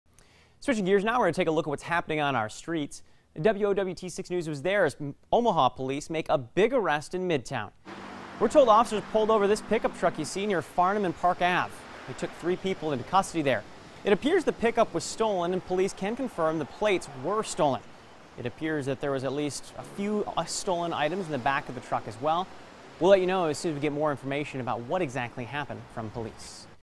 Three in Custody After Police Stop Stolen Pickup
Police stopped the pickup near Farnam Street and Park Avenue. It had stolen items in the truck bed.